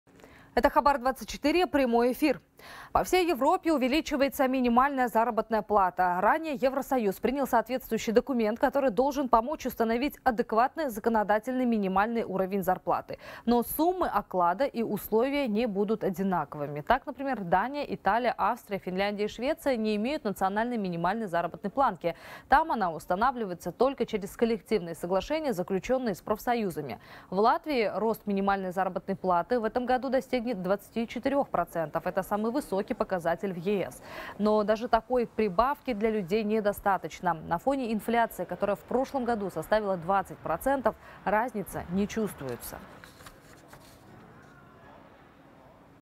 В ЕС повышают минимальную заработную плату
По всей Европе увеличивается минимальная заработная плата, сообщает корреспондент «Хабар 24». Ранее Евросоюз принял соответствующий документ, который должен помочь установить адекватный законодательный минимальный уровень зарпл